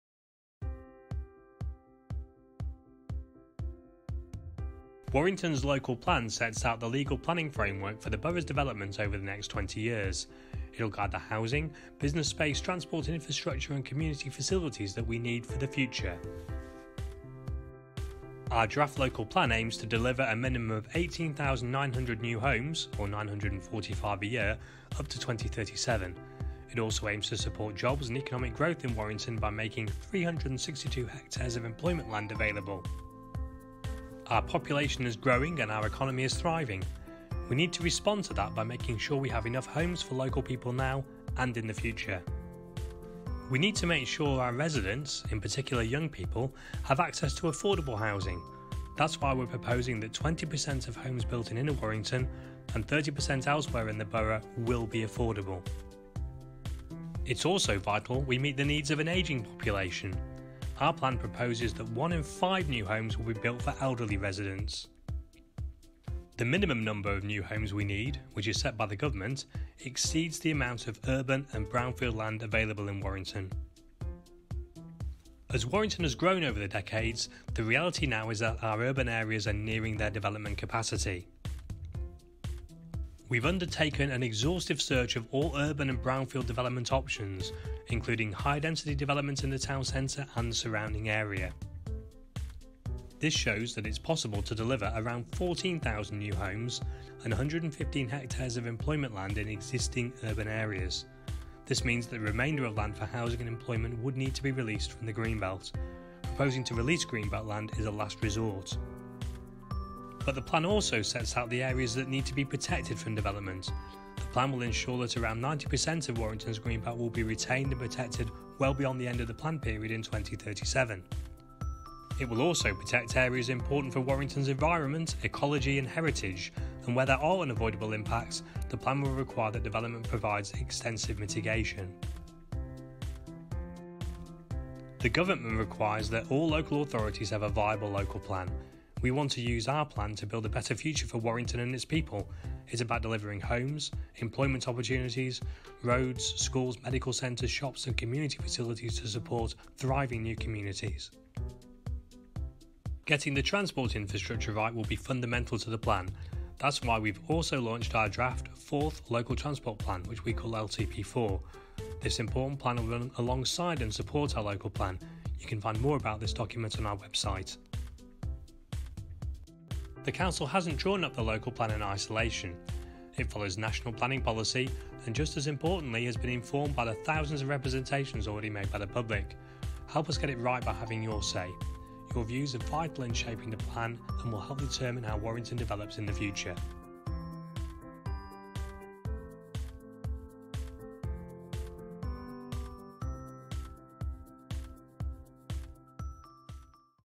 What is the Local Plan?
The Local Plan will shape, guide and influence how Warrington develops over the next 20 years...